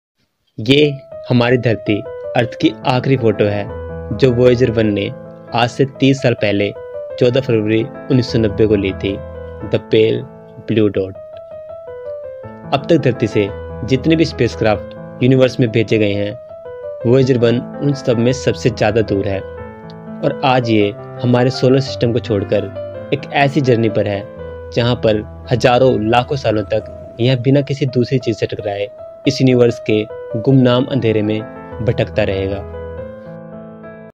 The Most Beautiful Image of Our Earth
What does Earth look like from space right now ?
Is there a photo of Earth from space?

The Blue Marble is an image of Earth taken on December 7, 1972, from a distance of around 29,000 kilometers (18,000 miles) from the planet's surface. Taken by the crew of the Apollo 17 spacecraft on its way to the Moon, it is one of the most reproduced images in history.

What is the last picture of Earth by Voyager?

Pale Blue Dot

Pale Blue Dot is a photograph of planet Earth taken on February 14, 1990, by the Voyager 1 space probe from a record distance of about 6 billion kilometers (3.7 billion miles, 40.5 AU), as part of that day's Family Portrait series of images of the Solar System